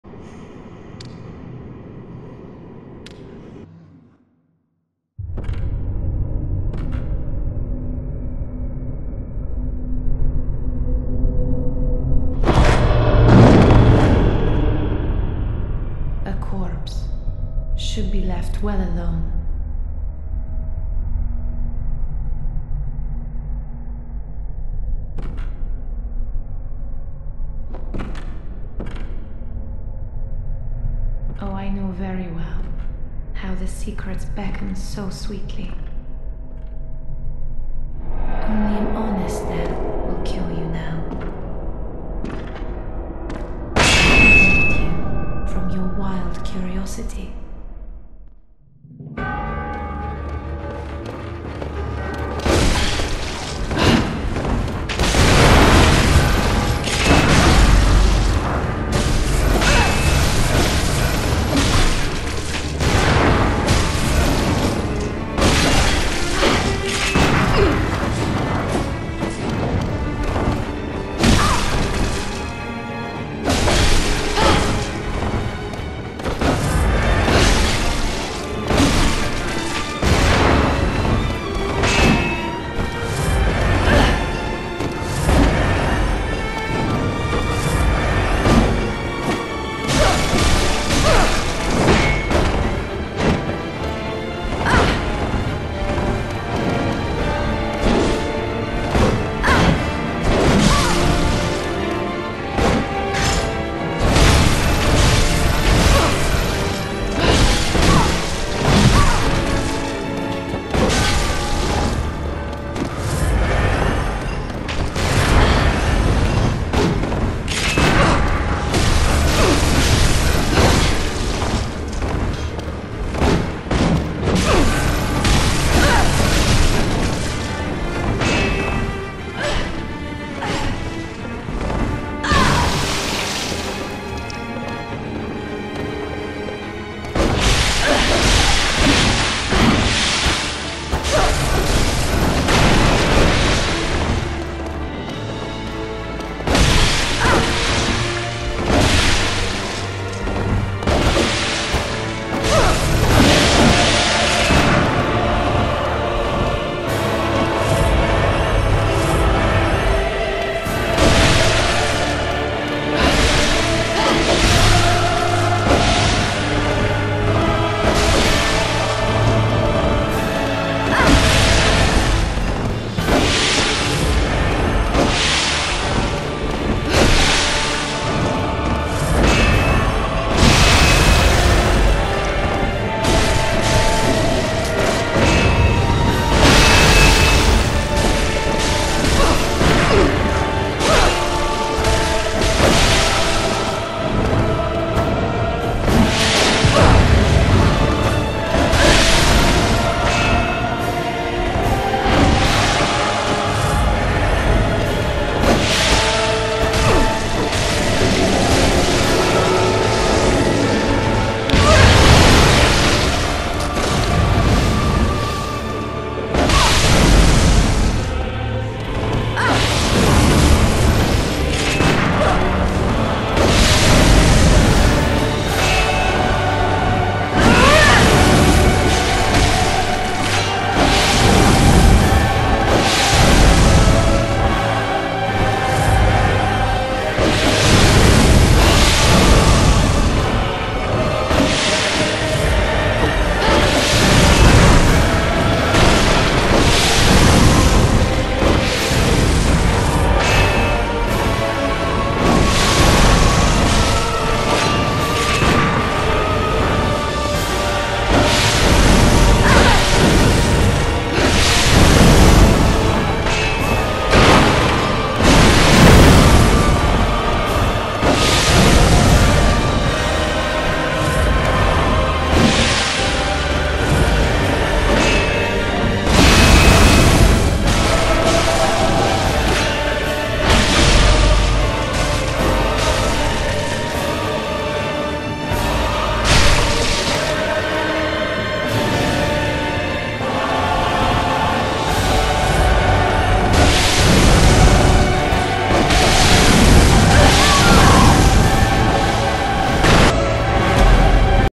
(Almost) First try at Maria (while drinking)
I was having fun and was not ✨sober✨ when I got to her soooo yeah I’m shocked I did so well.